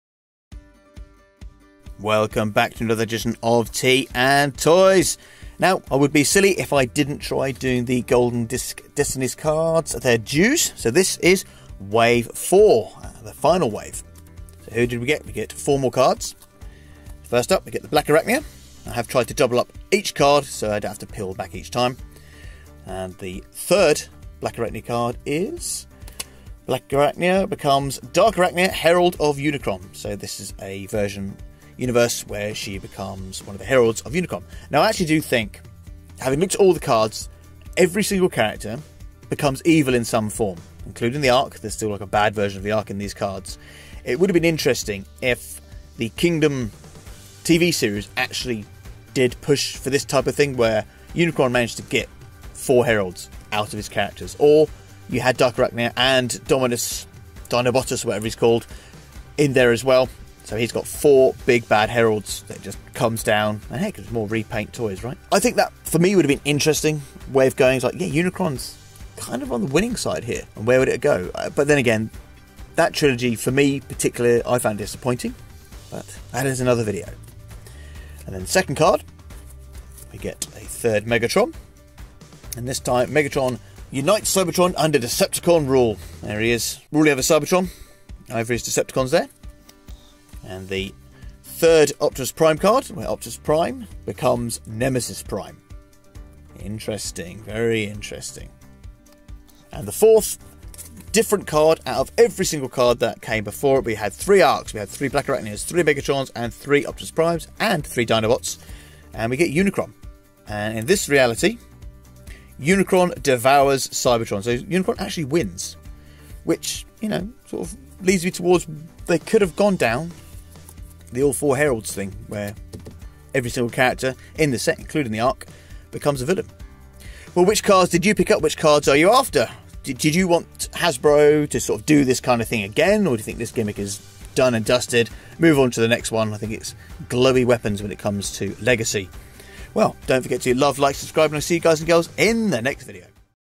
Transformers Kingdom Wave 4 Golden Disc Destiny Cards
nearly forgot about wave 4 before:

Blackarachnia - BECOMES DARKARACHNIA.. HERALD OF UNICRON
Optimus Prime - BECOMES NEMESIS PRIME
Megatron - UNITES CYBERTRON UNDER DECEPTICON RULE
Unicron - DEVOURS CYBERTRON